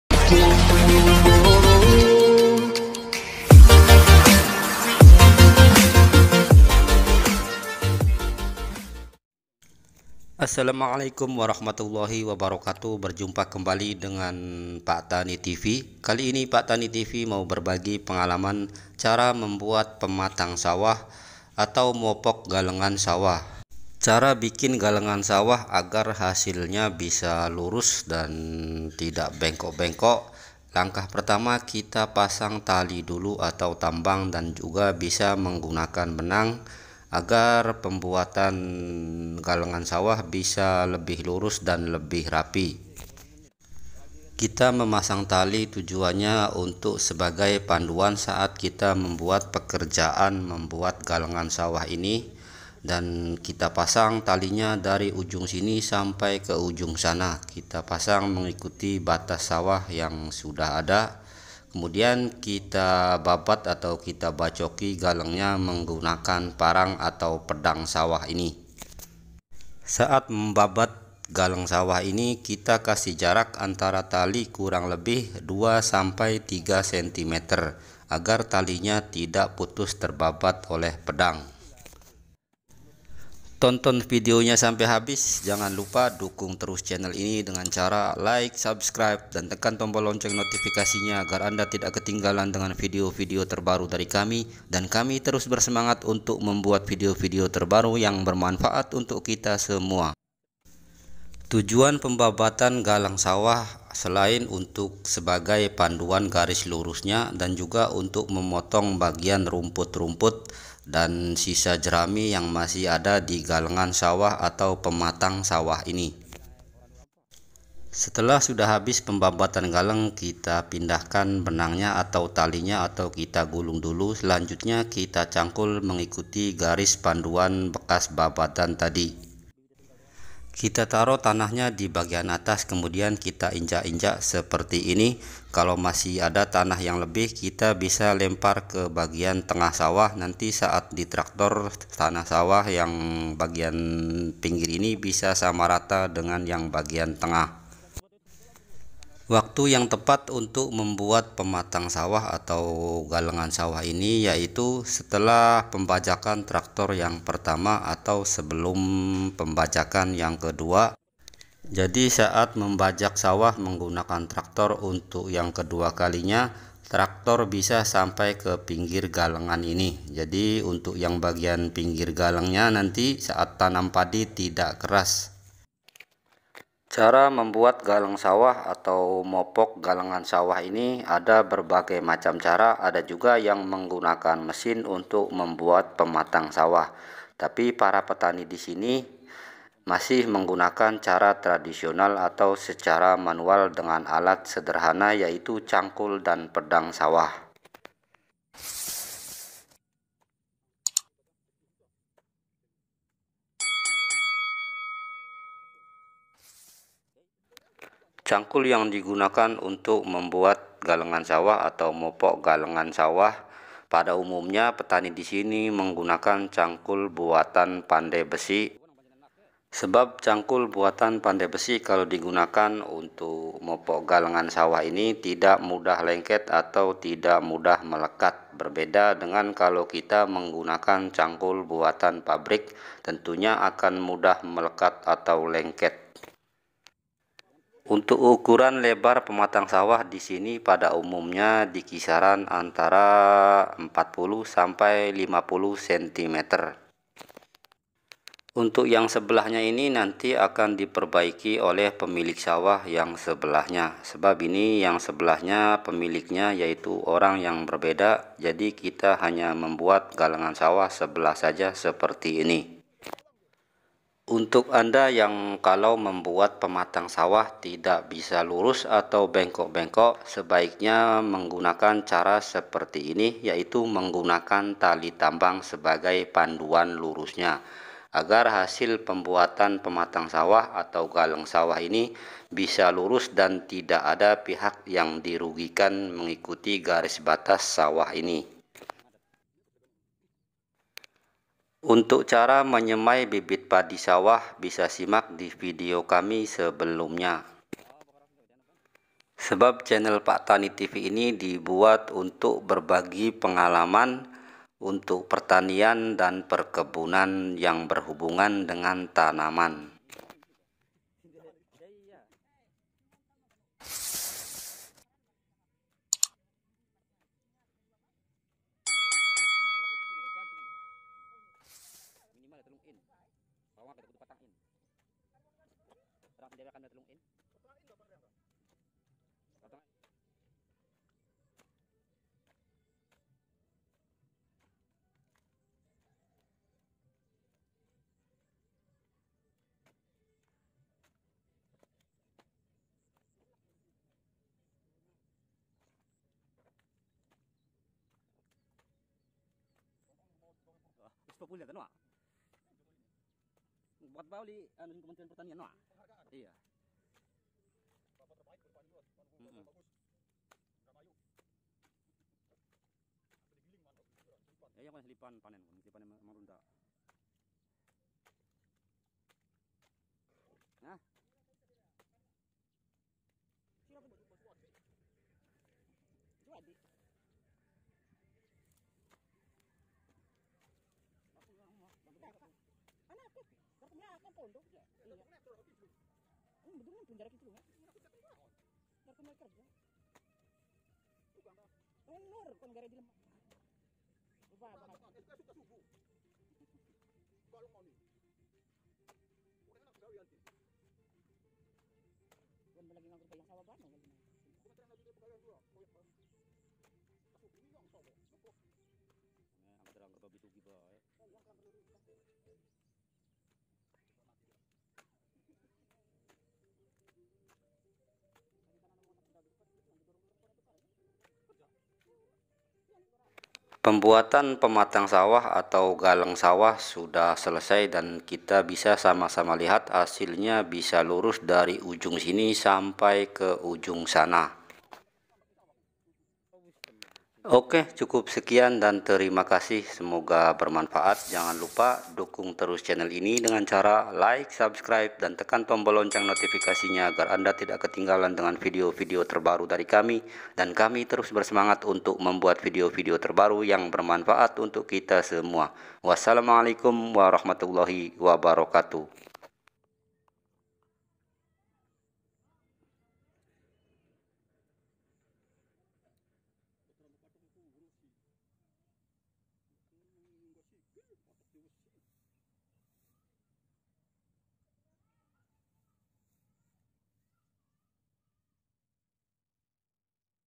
cara membuat pematang sawah ❗mopok galengan sawah
cara membuat pematang sawah/mopok galengan sawah, kali ini kita akan berbagi pengalaman cara membuat galeng sawah secara manual, alat yang digunakan cangkul, pedang sawah dan tali tambang, agar hasil pembuatan galengan sawah bisa lurus dan rapi,

semoga bermanfaat
terimakasih

sawah padi
pematang sawah
galengan sawah
galeng sawah
gegaleng sawah
mopok galeng
mopok galengan sawah
mopok galengan